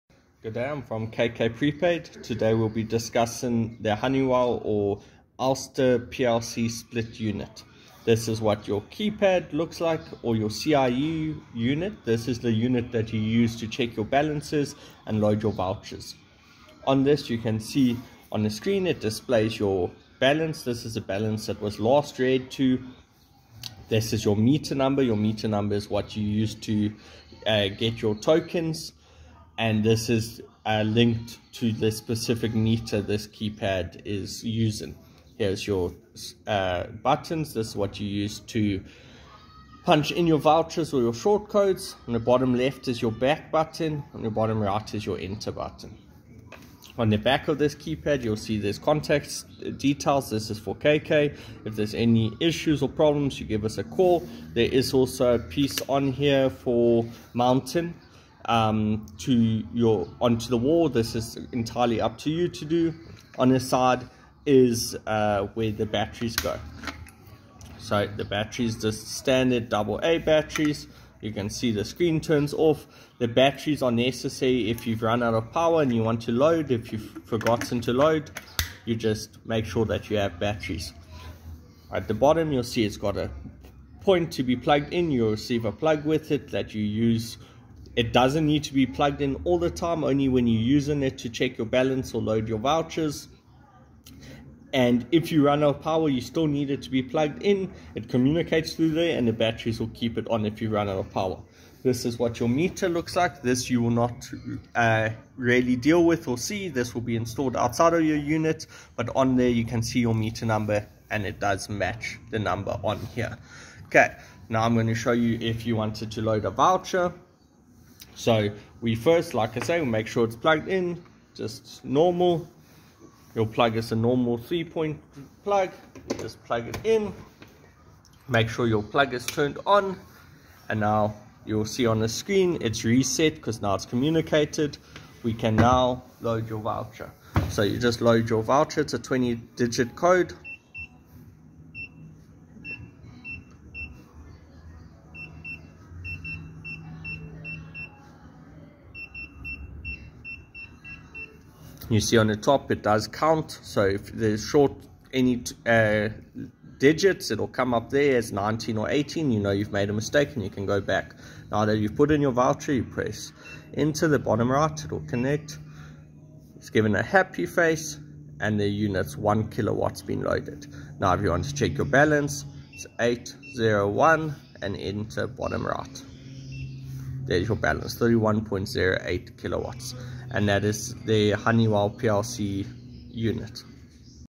KK Prepaid Honeywell / Elster Prepaid Meter
How The Honey Split PLC Prepaid Meter Looks And Works With CIU